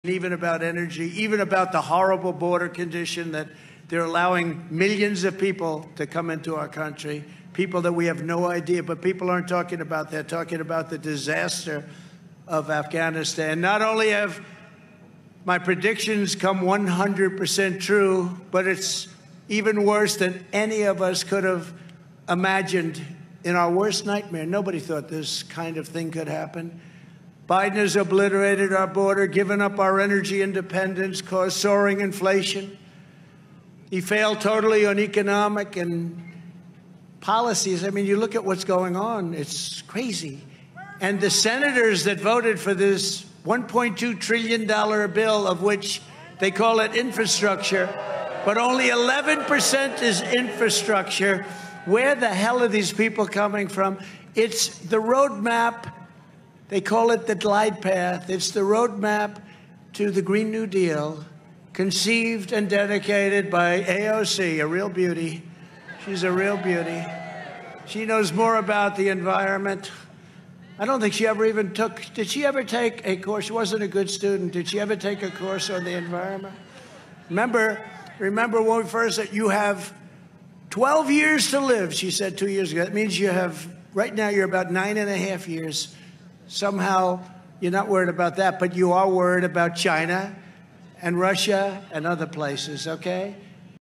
Trump Attacks AOC During Alabama Rally Speech
At his rally in Alabama, former President Trump took aim at Rep. Alexandria Ocasio-Cortez (D-NY) and the bipartisan infrastructure bill.